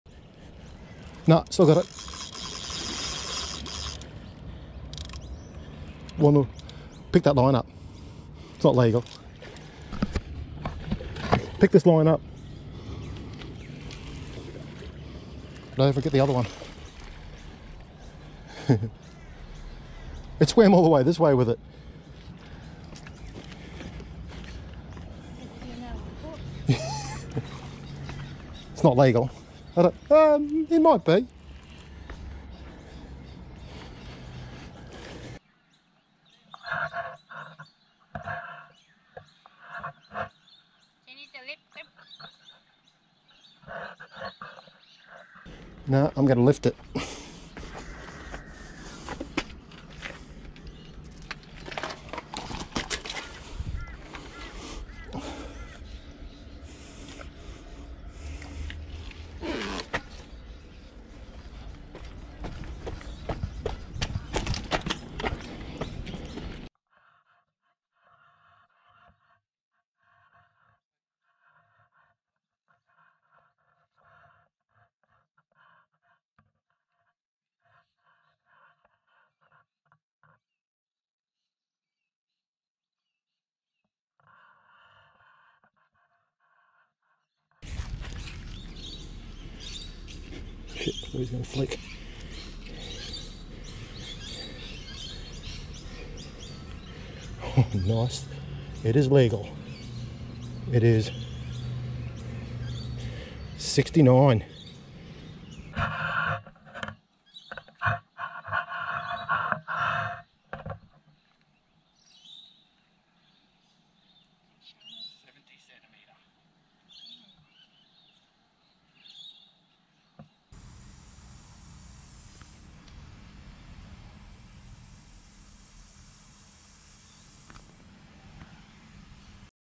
70 cm barra fitzroy river rockhampton
Fishing C.Q -Rockhampton,Yeppoon,Causeway Lake

-Penn FIERCE III 8000 LIVE LINER
-Rod Ugly stik Line weight 10 to 20 kg light action
-30 lbs  platypus
-60 lbs  Fluorocarbon Leader

-Penn FIERCE III 6000 LIVE LINER
-Rod Ugly stik Line weight 5 to 8kg light action
-15lbs/7kg Maxima Ultragreen
-20lbs Shimano Ocea EX Fluorocarbon Leader

-Reel Shimano Baitrunner 4000 D
-Rod Ugly stik Line weight 3 to 5kg light action
-8lbs/3.6kg Berkley Trilene
-20 lbs Shimano Ocea EX Fluorocarbon Leader

-Reel Shimano Baitrunner 6000 OC
-Rod Ugly stik Line weight 3 to 7kg light action
-15lbs/7kg Maxima Ultragreen
-20 lbs Shimano Ocea EX Fluorocarbon Leader

-Kayak used is the  Dragon Pro KingFisher

-Super wide 1080/60